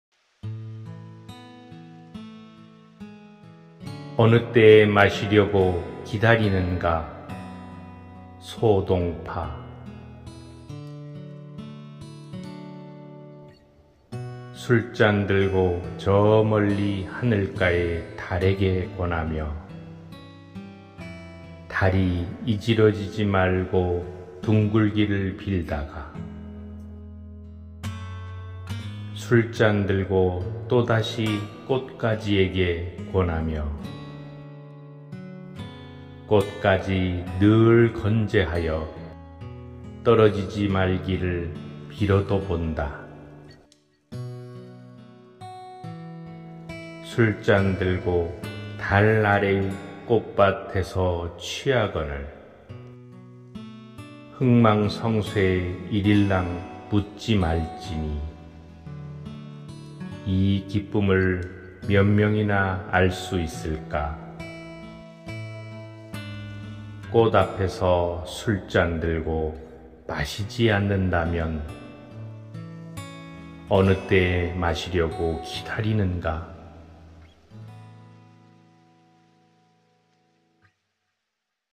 임감독의 풍경 ㅡ어느때에 마시려고 기다리는가(소동파)
시 소동파(본명 소식)
낭송 임성수
음원 ㅡ키네마스터 유료구독